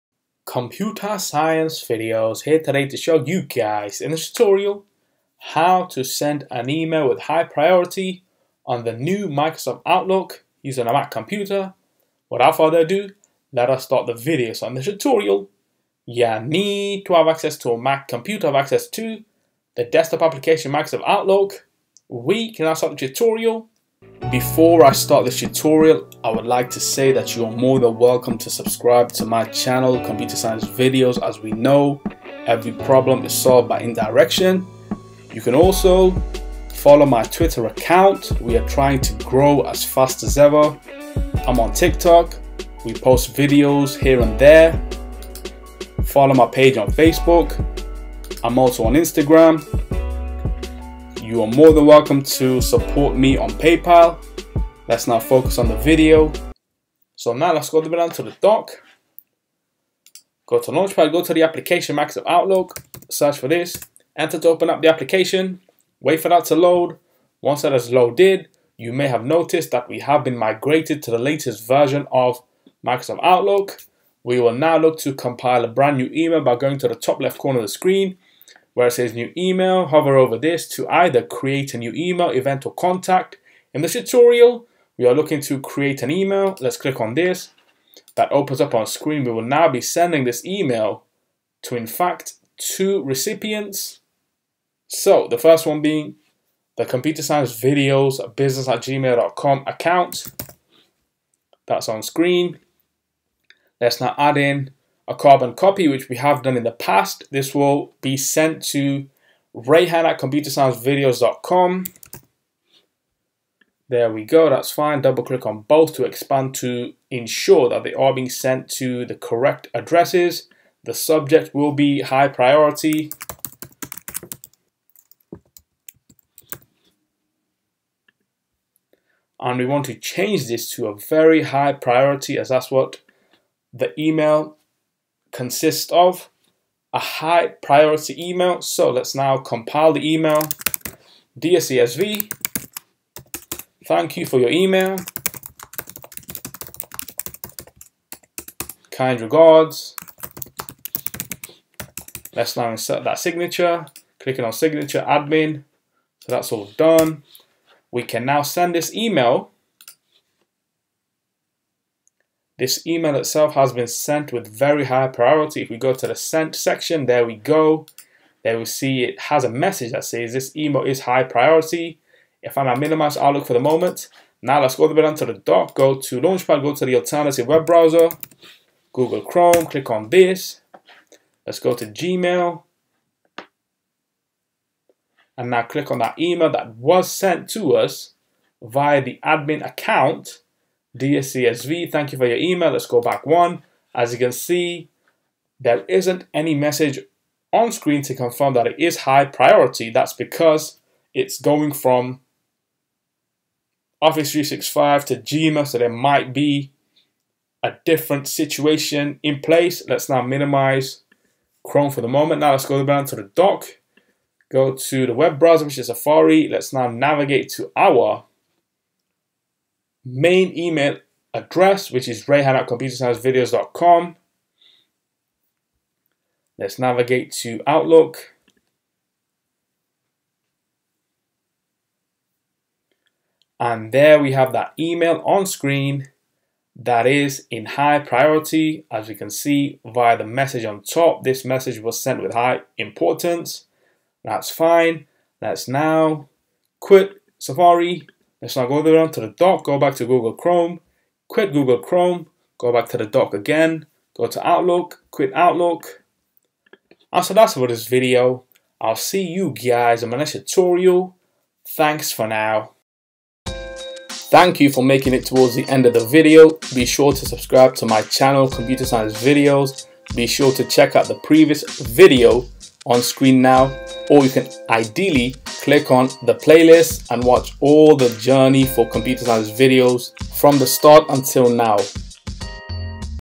How to SEND an Email w/ High Priority On The New Microsoft Outlook Using a Mac Computer - Tutorial
How to SEND an Email w/ High Priority On The New Microsoft Outlook Using a Mac / Desktop Computer - Basic Tutorial | New